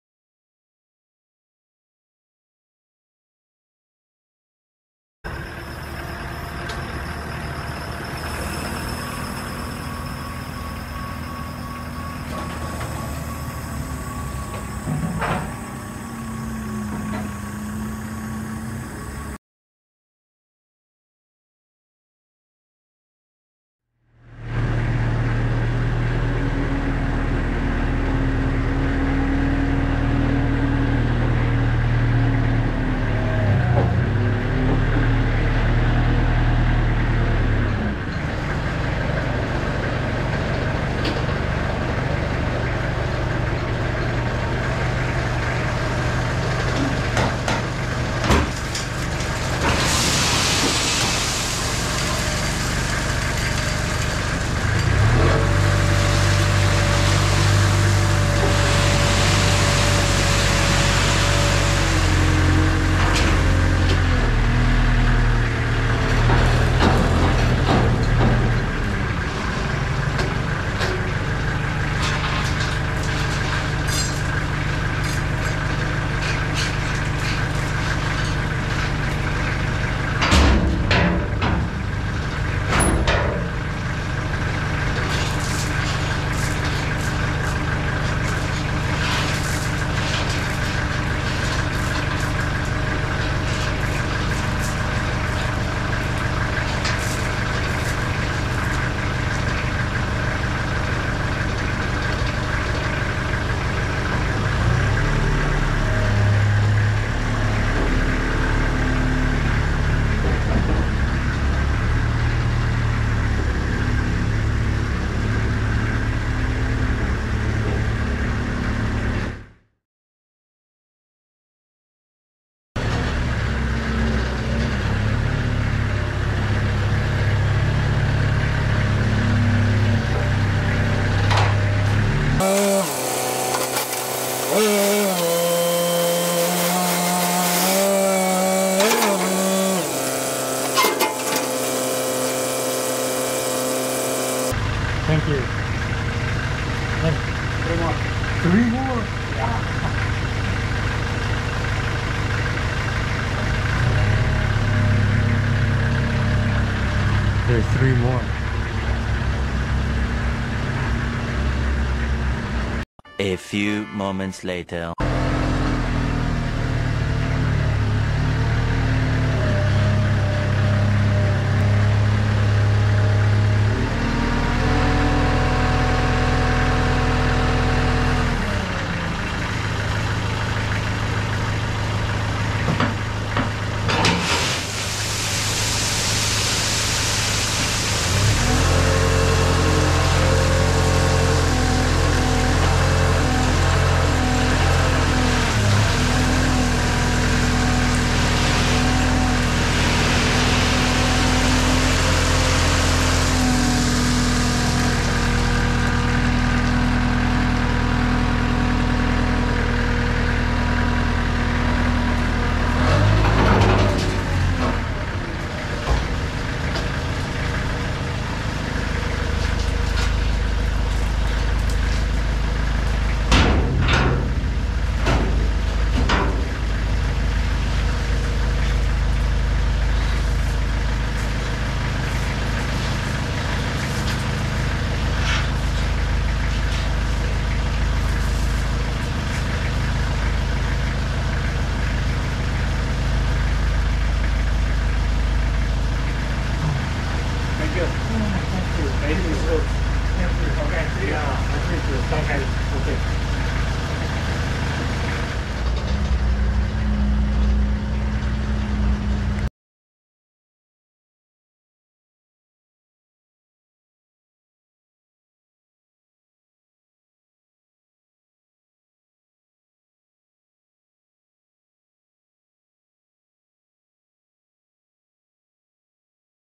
Patagonia Landscape Supply rock delivery
Delivery of crushed rock from Patagonia Landscape Supply, Inglewood Ca.
160 square feet, $630 delivered.